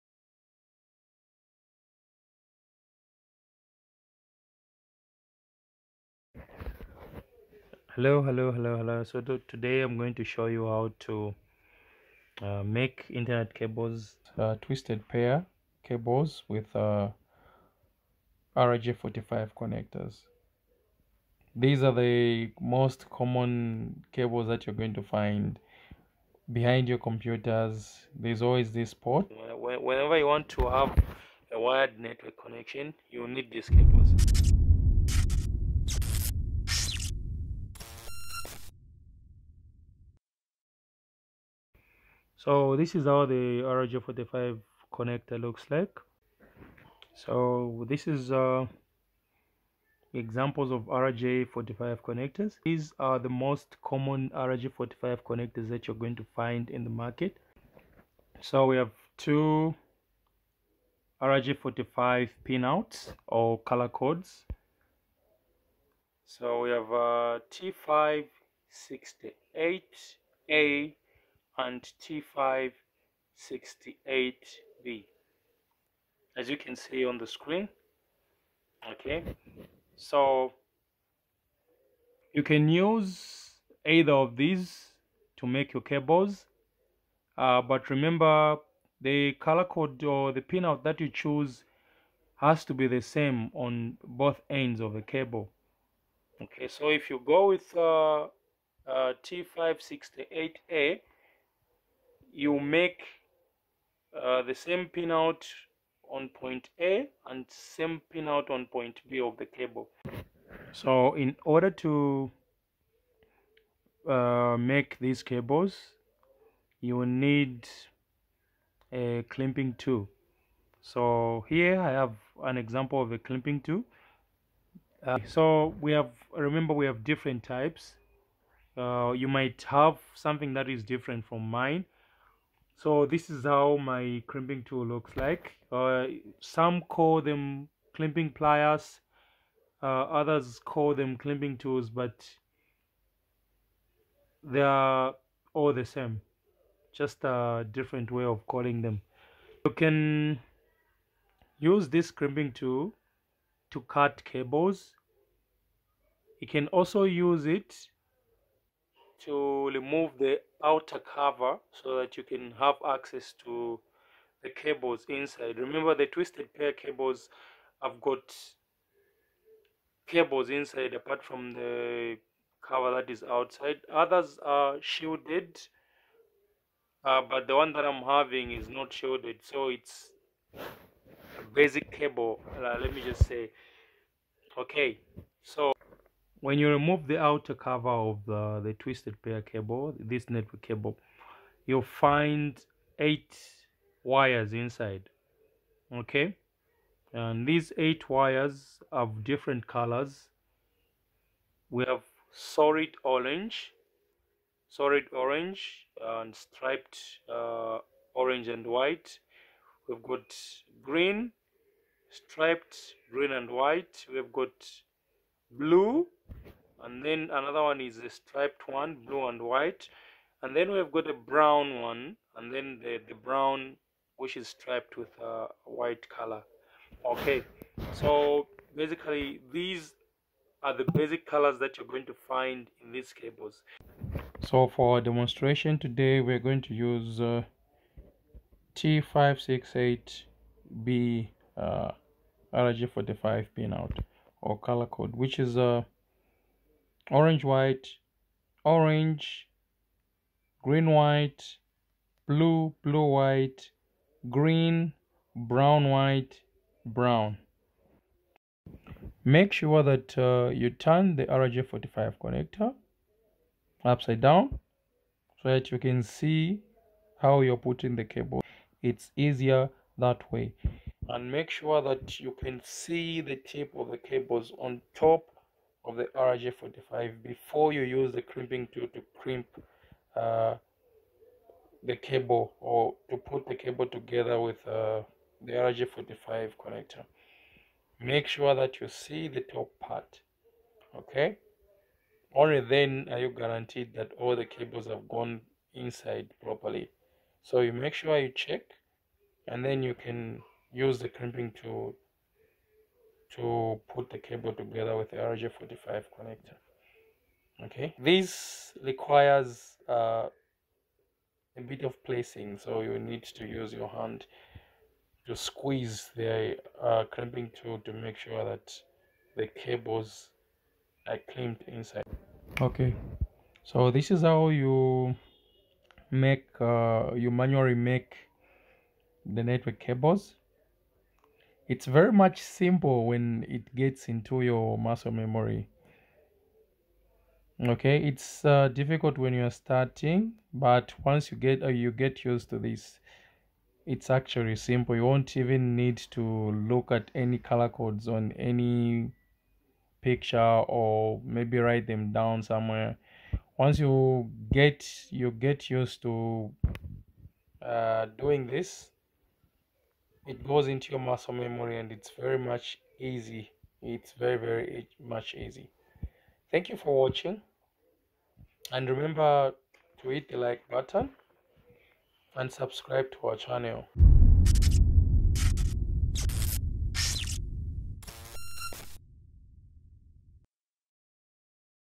how to make internet cable with RJ45 connector
the easiest way to make twisted pair internet cables manually with RJ45 connectors. with this video you will easily learn how to climp/attack the RJ45 connnector to a twisted pair cable...